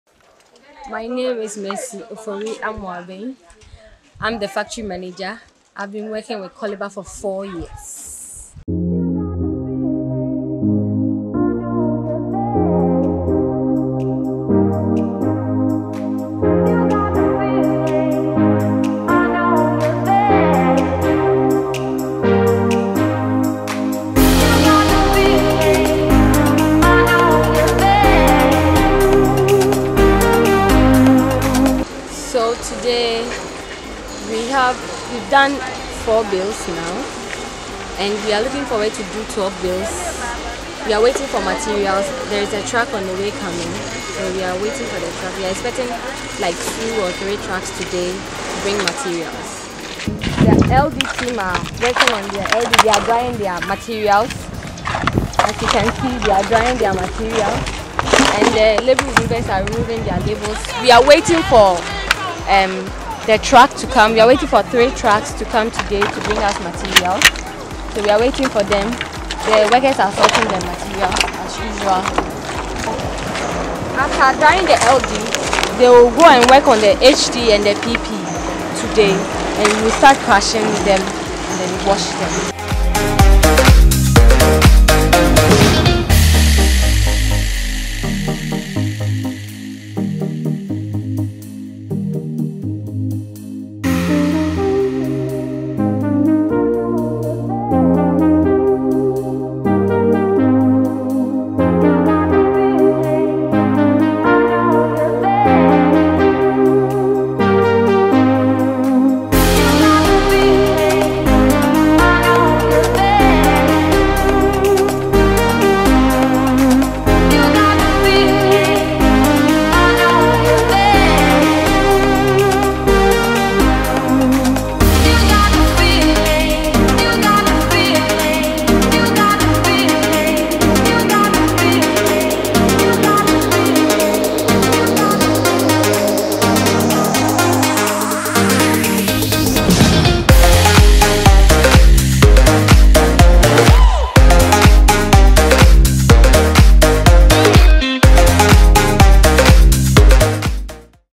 Coliba Ghana Documentary
COLIBA is an African start-up that puts technology at the service of recycling plastic waste through a Web application, Mobile, and SMS.